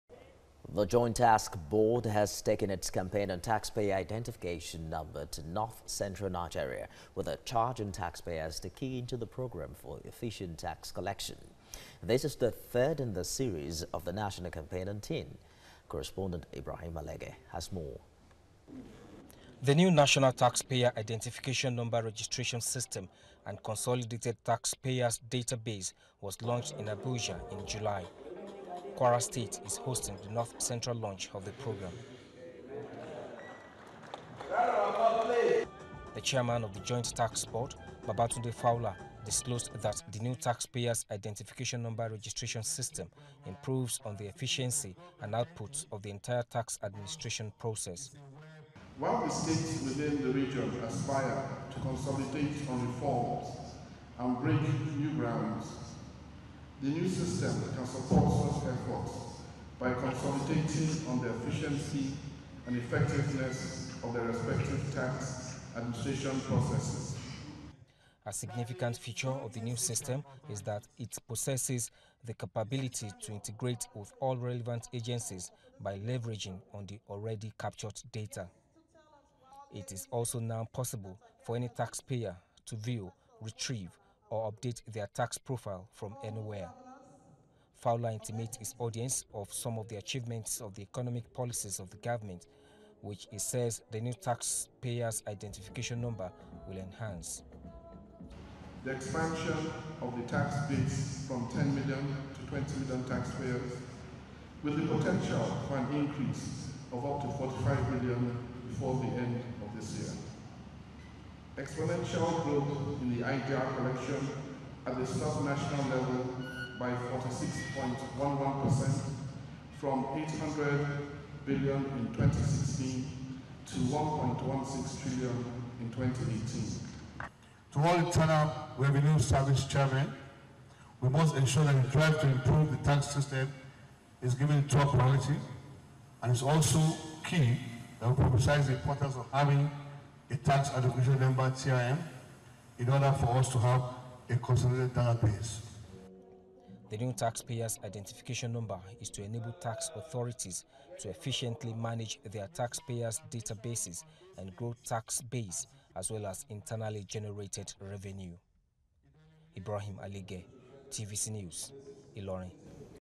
JTB launches tax payers registration in north central
Joint Tax Board has commenced tax payers registration in North central to improve on tax remittance in the region.